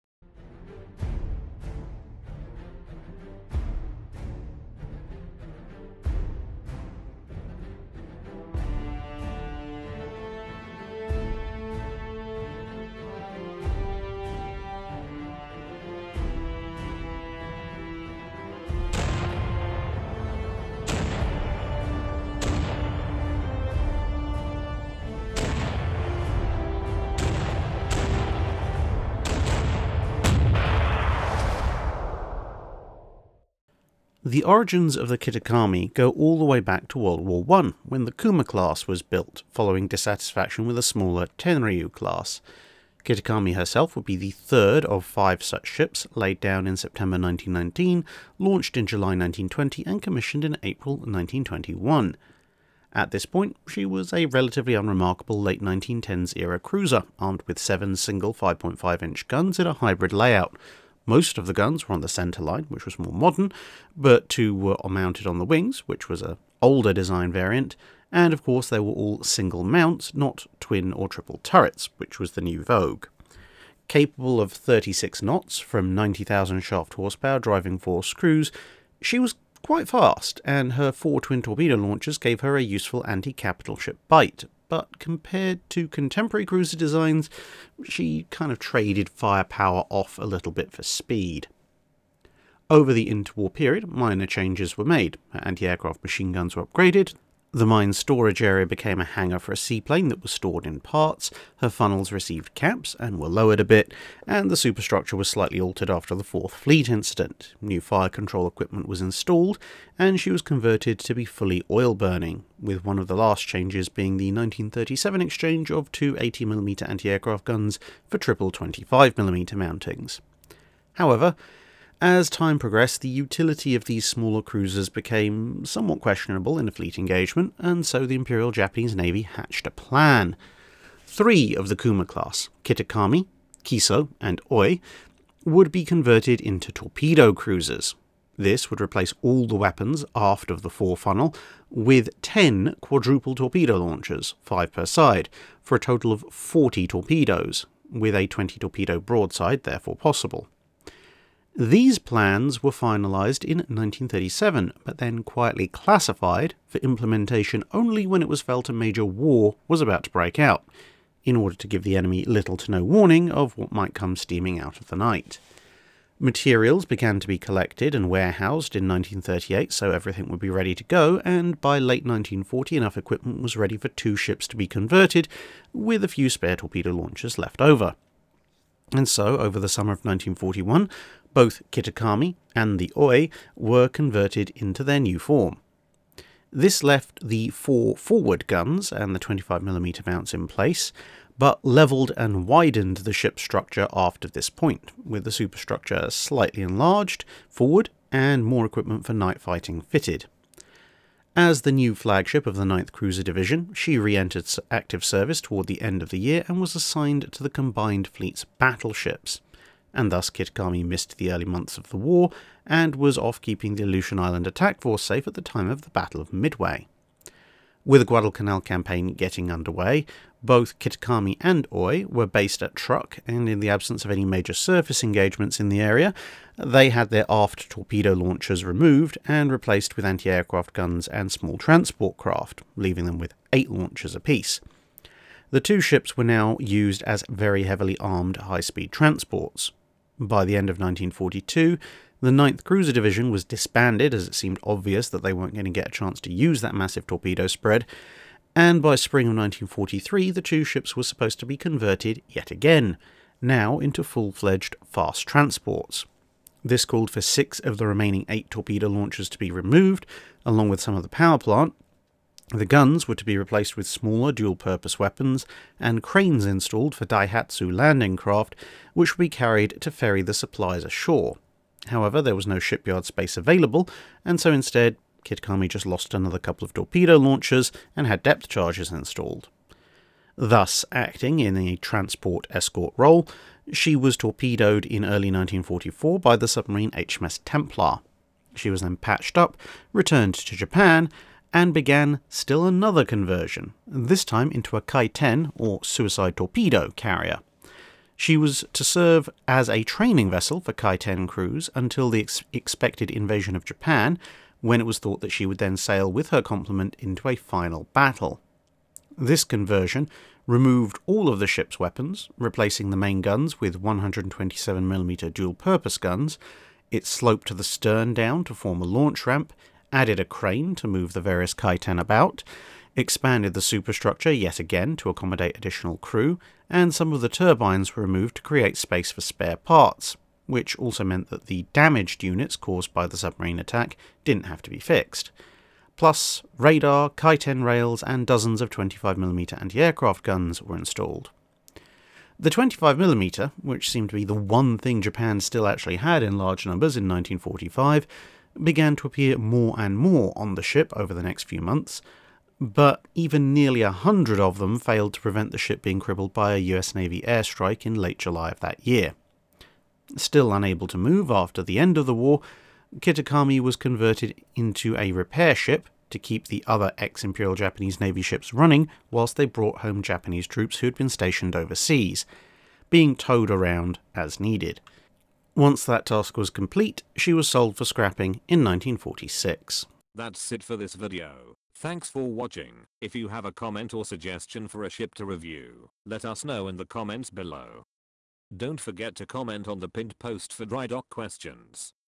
IJN Kitakami - Guide 285
The Kitakami, a torpedo cruiser of the Imperial Japanese Navy, is today's subject.

Read more about the ship here:
Kitakami, by Hans Lengerer, Sumie Kobler-Edamatsu and Tomoto Rehm-Takahara - Warship Volume X